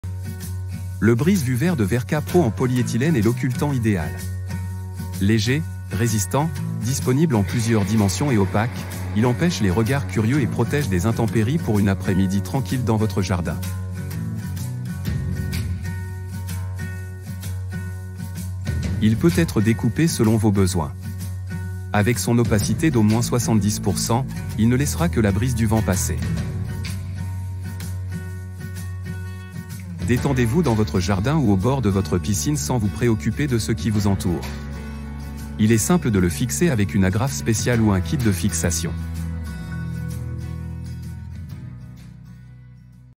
🌿 Découvrez le Brise-Vue Vert de WERKA PRO : Votre Oasis de Tranquillité dans le Jardin ! 🌞
🔹 Le secret d'un jardin paisible avec provence-outillage.fr  🏡

Vous rêvez d'une après-midi tranquille dans votre jardin, à l'abri des regards indiscrets et des intempéries ? Le brise-vue vert de WERKA PRO est là pour réaliser votre souhait. Fabriqué en polyéthylène, il est à la fois léger et résistant, offrant une protection durable pour votre espace extérieur.

🔹 Opacité et protection 💨

Avec une opacité d'au moins 70%, ce brise-vue vous offre l'intimité dont vous avez besoin tout en laissant passer la brise rafraîchissante du vent. Profitez de moments de détente sans souci, que ce soit dans votre jardin ou au bord de votre piscine.

🔹 Installation facile et polyvalente 🔧

L'installation de ce brise-vue est un jeu d'enfant. Fixez-le rapidement avec une agrafe spéciale ou un kit de fixation, et voilà ! Votre oasis de tranquillité est prête à vous accueillir.

🔹 Profitez dès maintenant de votre espace extérieur en toute sérénité avec le Brise-Vue Vert de WERKA PRO ! 🌿

Découvrez-le dès aujourd'hui dans notre magasin ou sur notre site web. 🌼✨